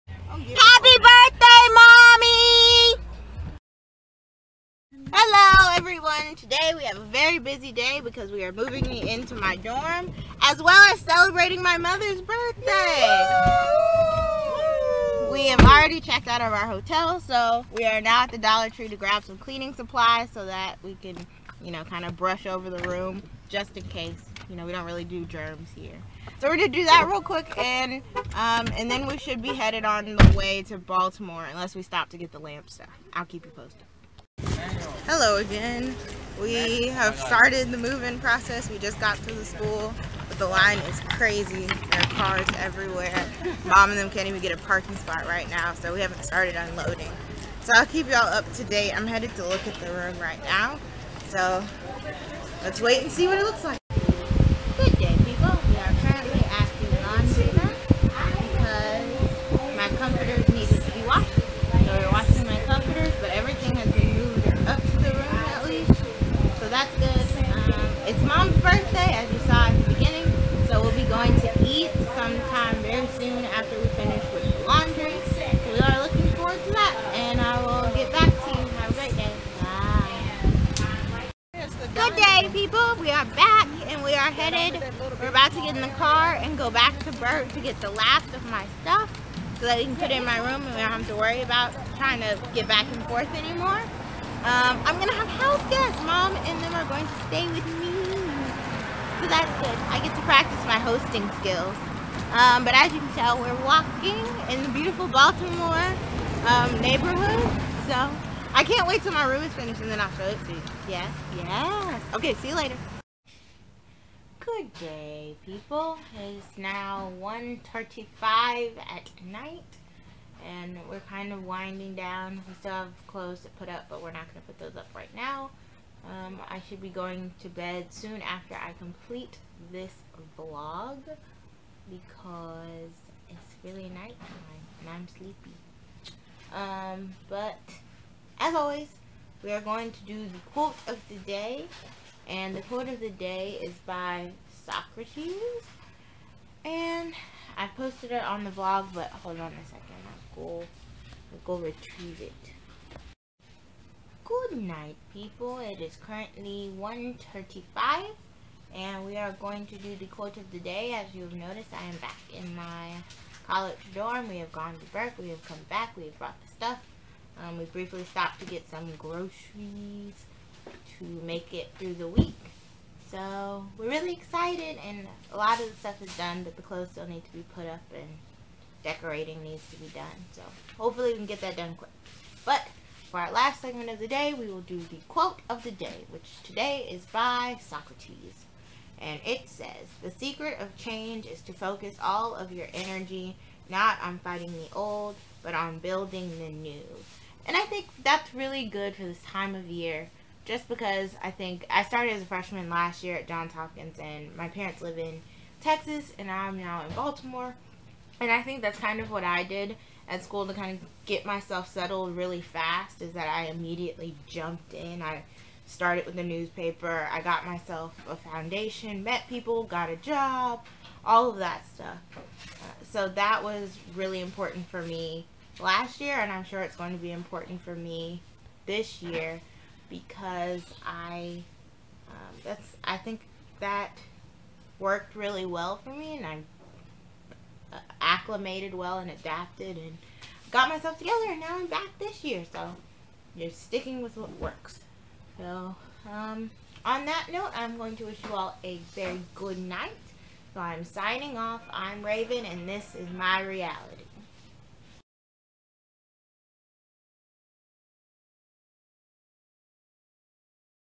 Raven's Eyeview on New Beginnings (Day 6)
Raven and crew move into her dorm!